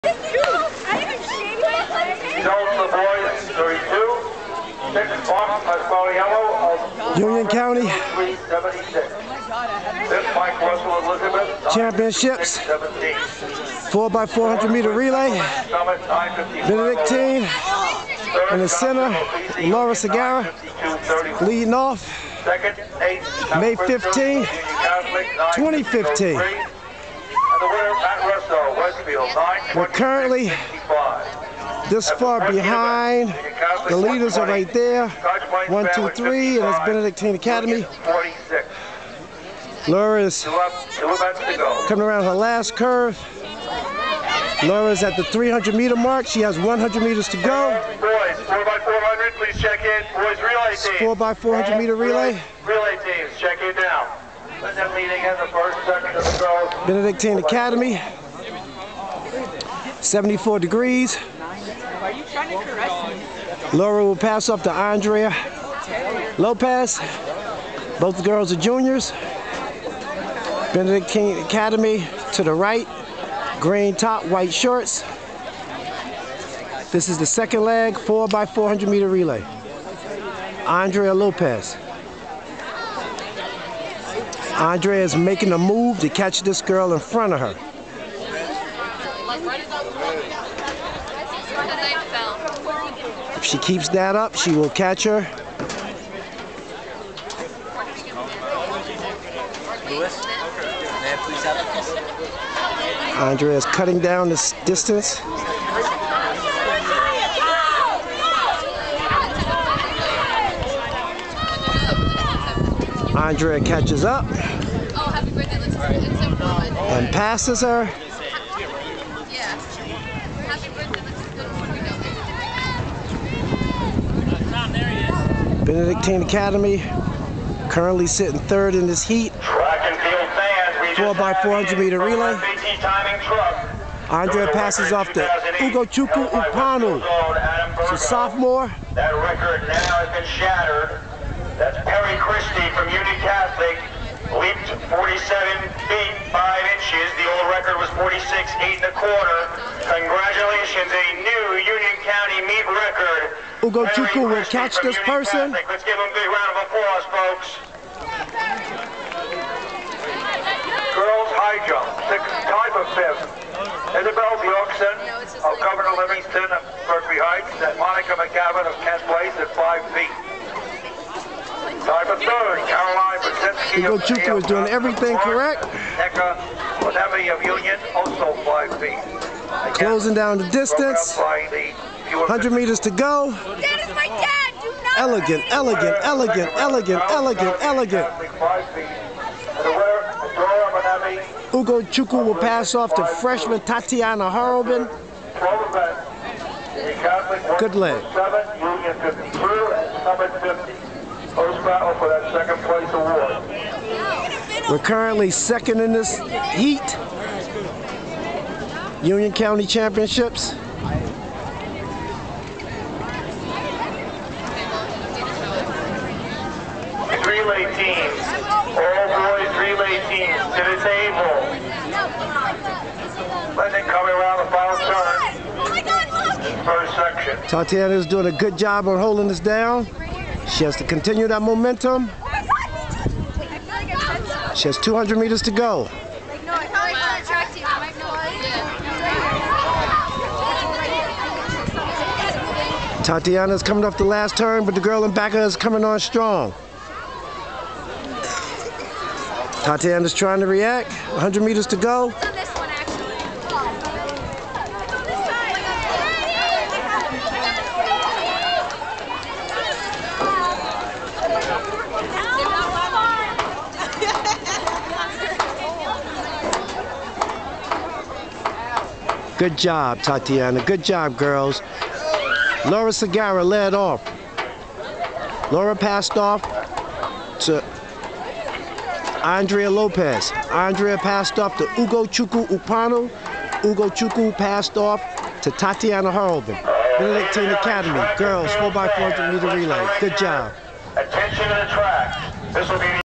U.C.I.A.C. Champ.,5-15-2015, B.A.,4x4MR, L.Segarra,A.Lopez, U.Ukponu,T.Horrobin ByANC
U.C.I.A.C. Champ.,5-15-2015, Benedictine Academy,Eliz.,NJ. 4x400 Meter Relay,1- Laura Segarra-65.67, 2-Andrea Lopez-67.17, 3- Ugochukwu Ukponu-64.55,4th-Tatyana Horrobin-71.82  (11th of 13), 4:29.34.Huib Stine Field Plainfield, NJ. By Angelo Norris Clark